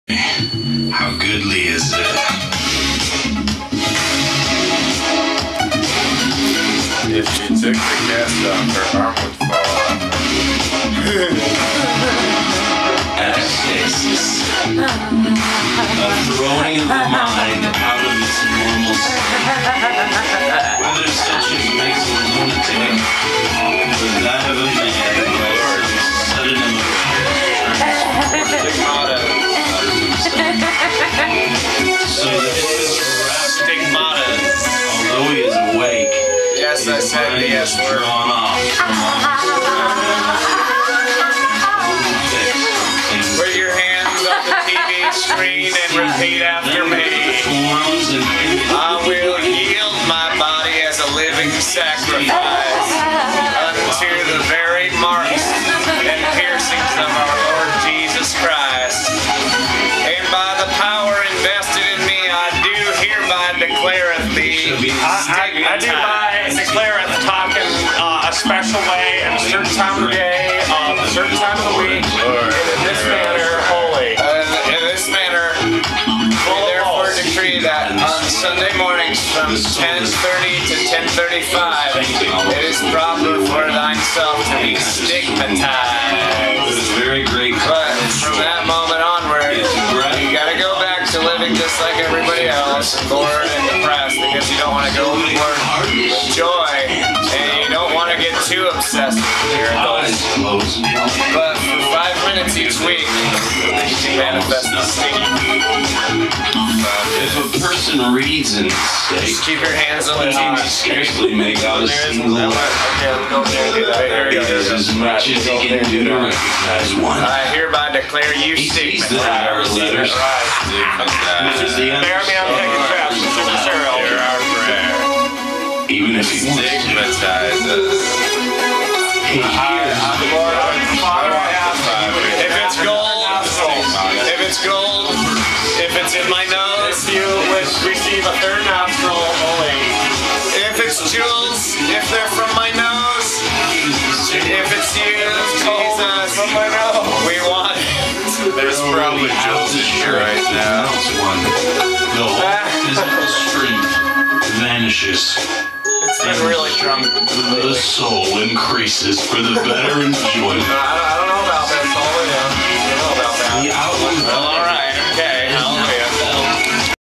A Quick Stiggie Impartation!!
Historically, the invisible stiggie comes first. Can you feel the pressure starting on your hands, feet, or forearms? But, be careful manifesting just anywhere, we wouldn't want to become obsessive with it or anything.

P.S. Sorry about the watermark, we too drunk to process it and stufffffffffffffffffffffffffff